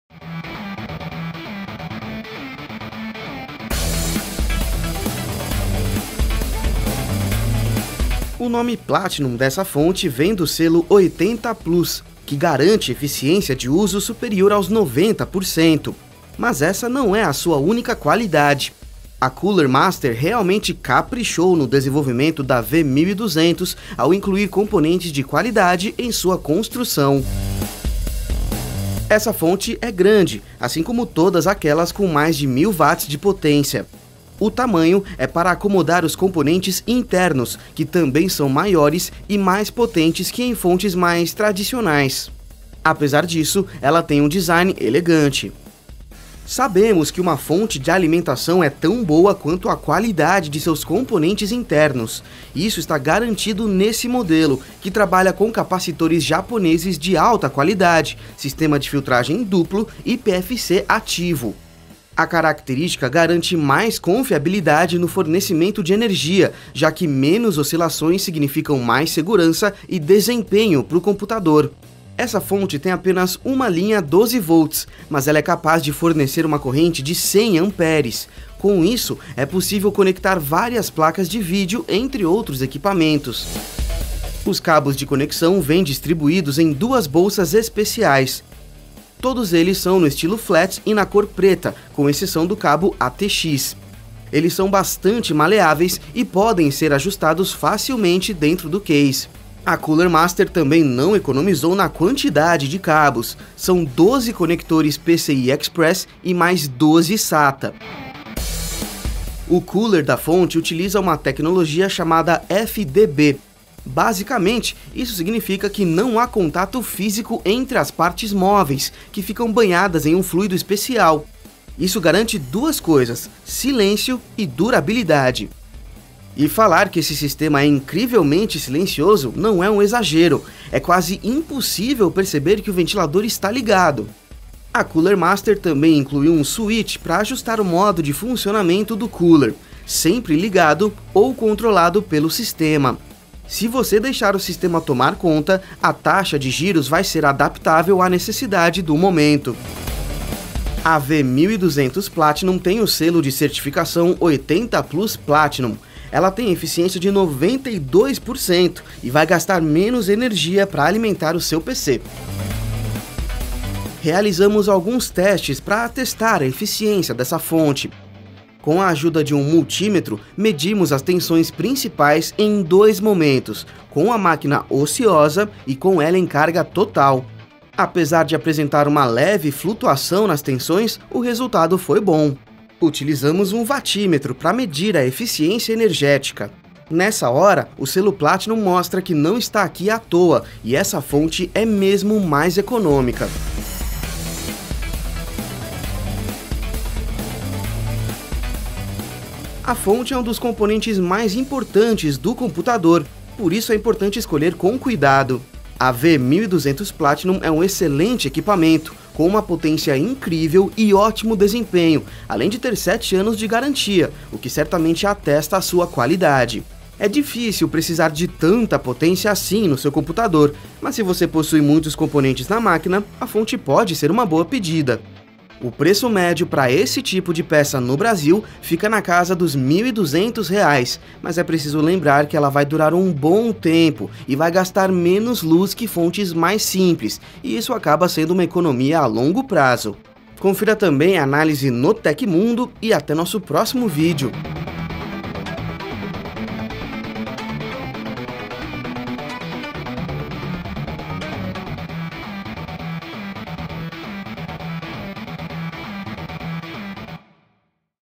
Fonte Cooler Master V1200 Platinum [Análise de Produto] - TecMundo
O nome Platinum dessa fonte vem do selo 80 Plus que garante eficiência de uso superior aos 90%. Mas essa não é a sua única qualidade. A Cooler Master realmente caprichou no desenvolvimento da V1200 ao incluir componentes de qualidade em sua construção.